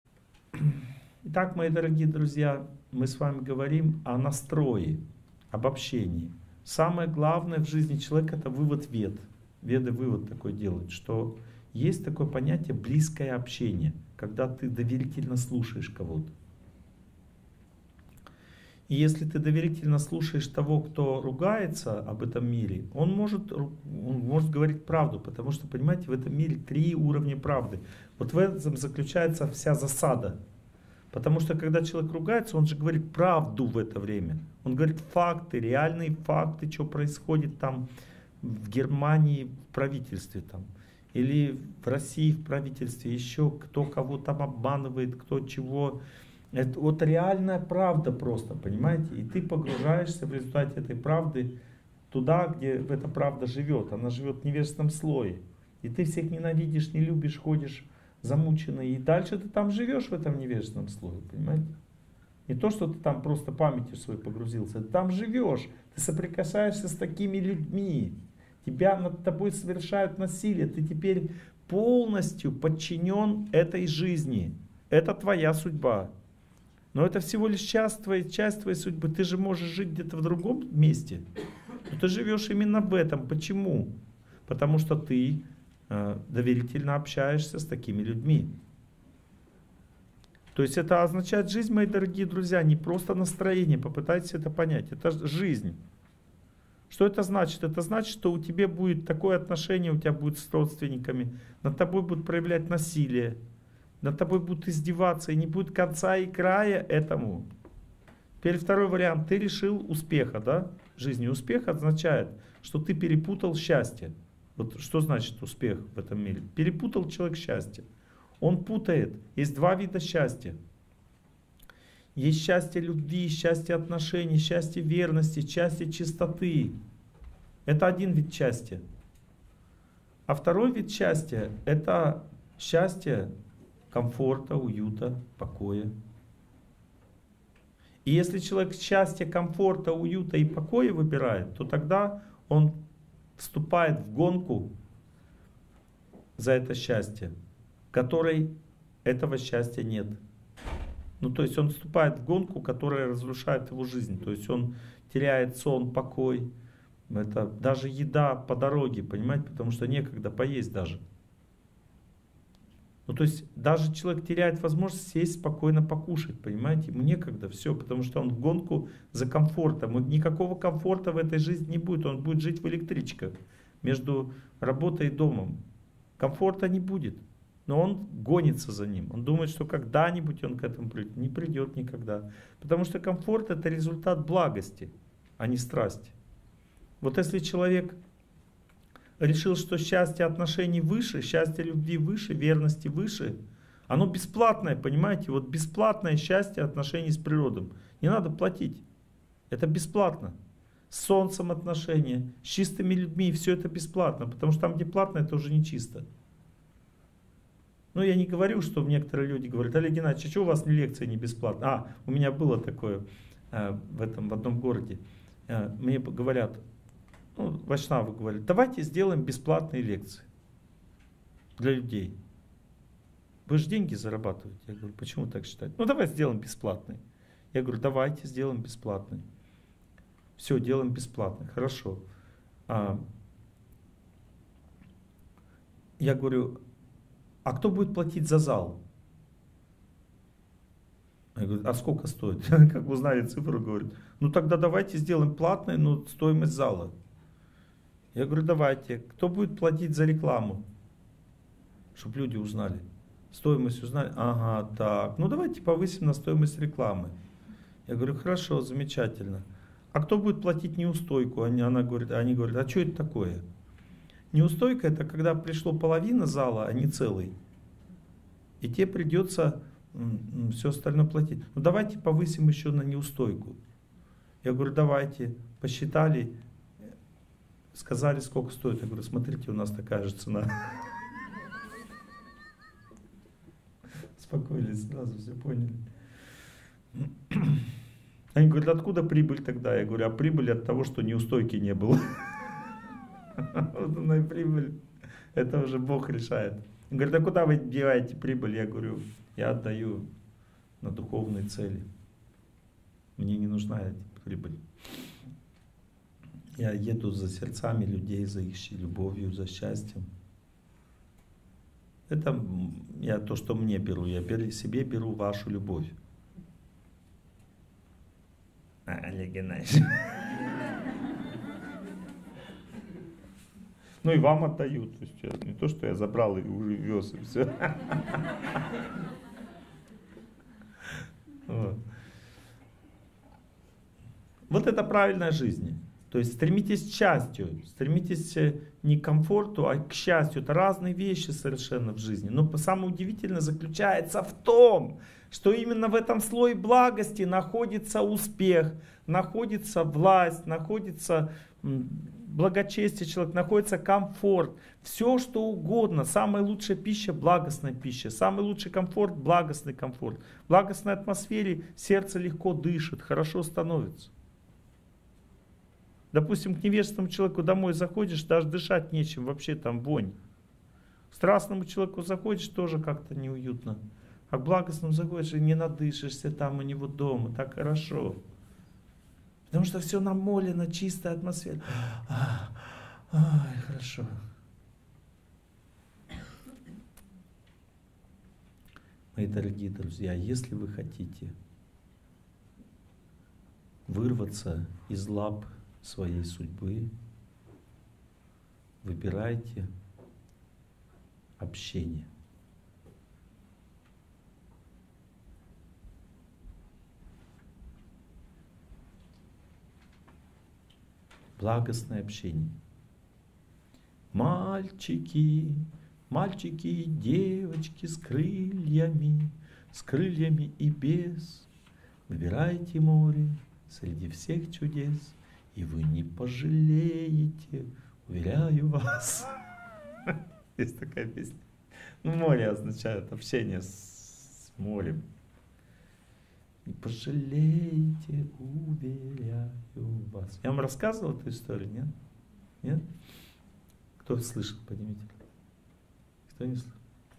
Торсунов О.Г. Доверительное общение
В этом видео Олег Геннадьевич Торсунов говорит о доверительном общении
Из лекции « Сила чувства собственного достоинства »,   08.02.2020 Бад-Эйнхаузен, Германия